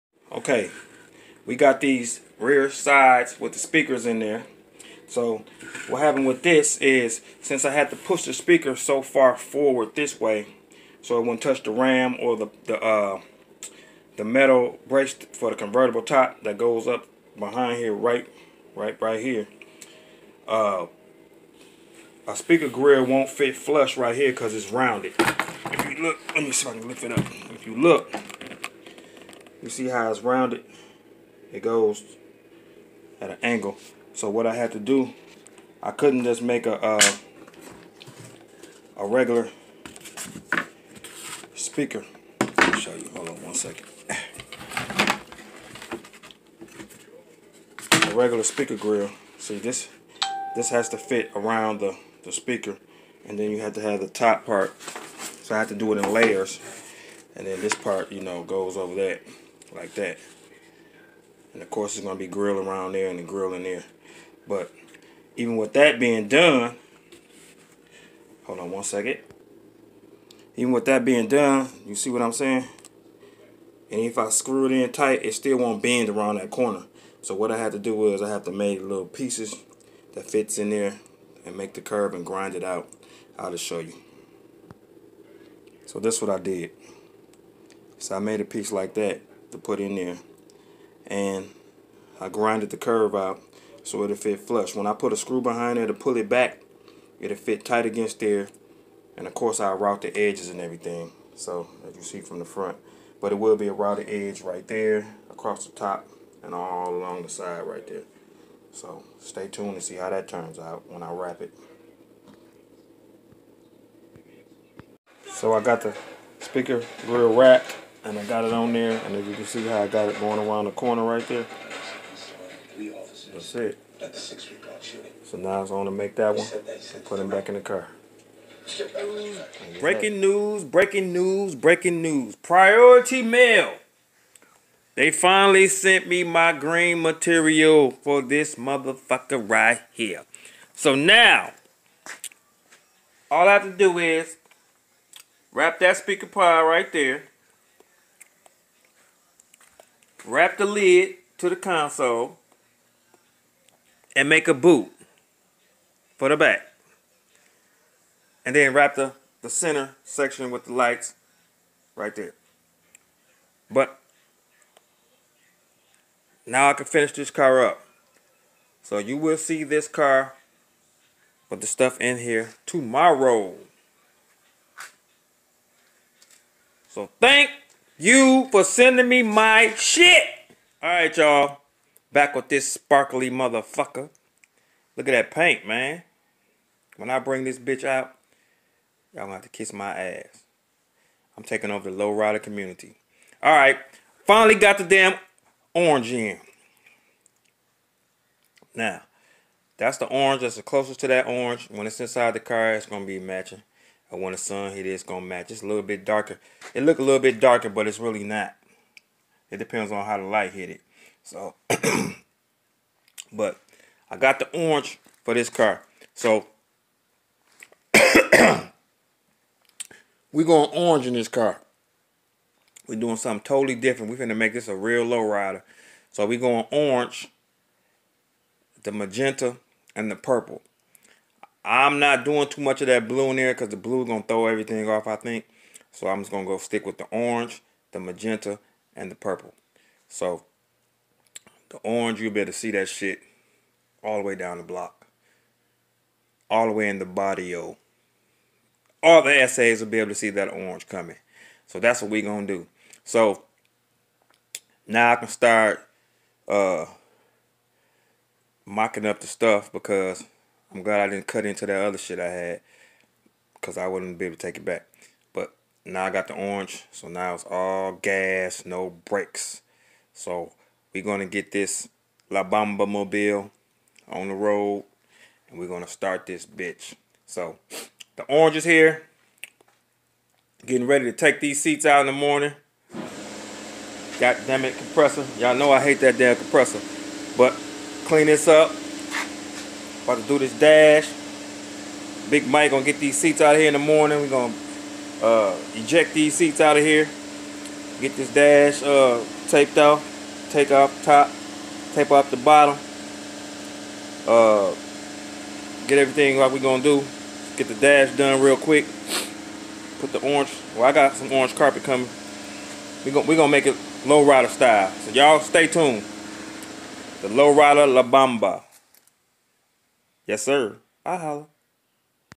67 Skylark speaker pod install
Explaining the problems qith the speaker location